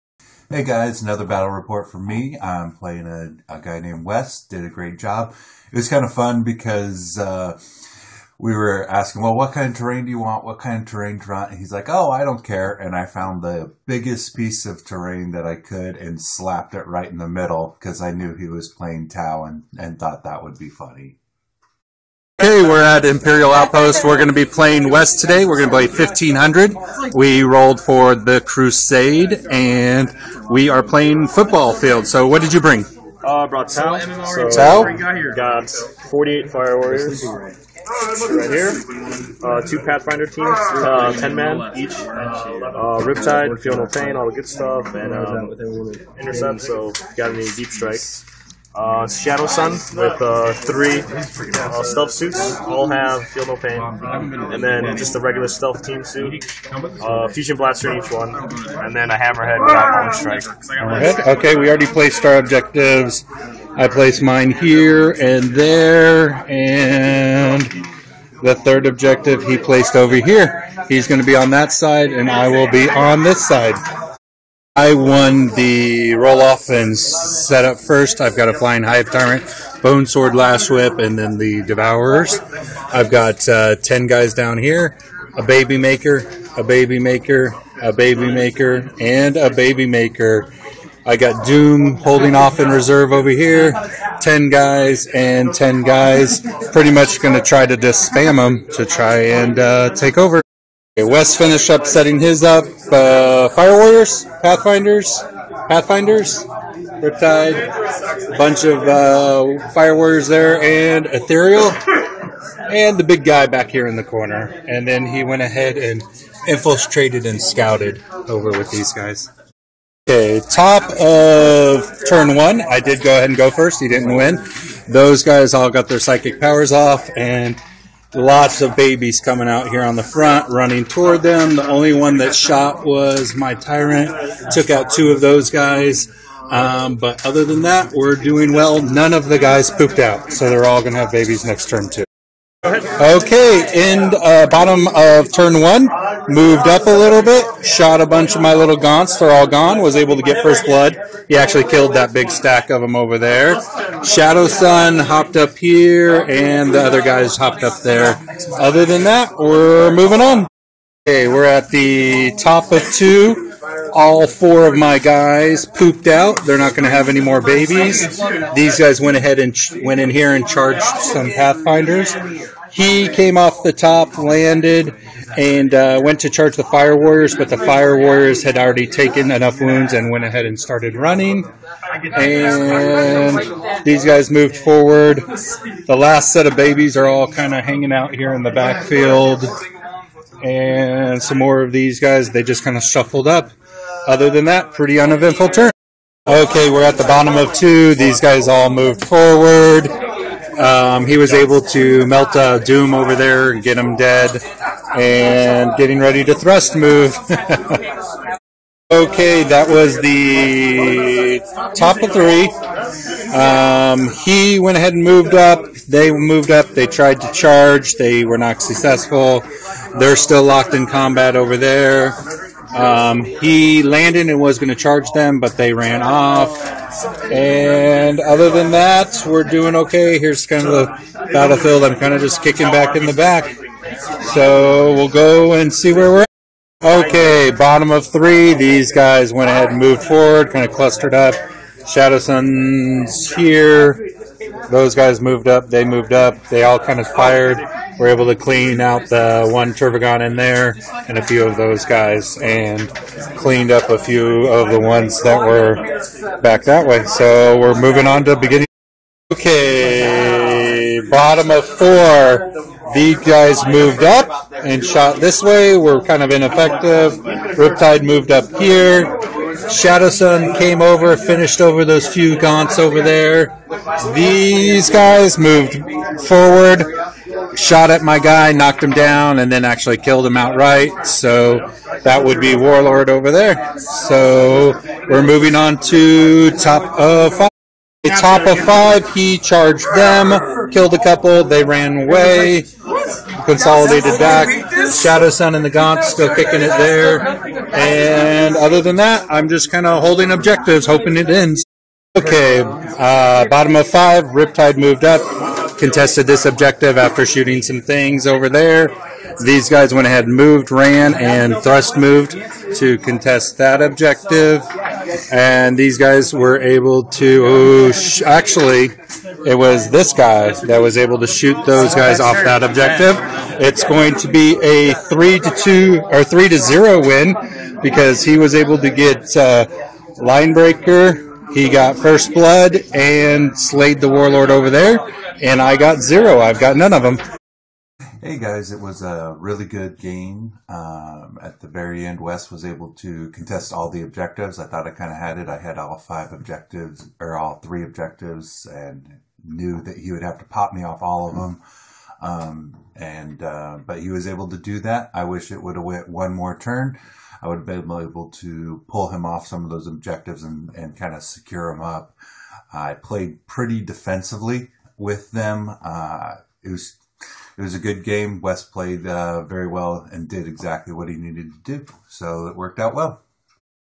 Warhammer 40k Battle Report 1500 pts Tau Vs Tyranids
Wes and I playing a 1500 pt game, watch and see how it turns out.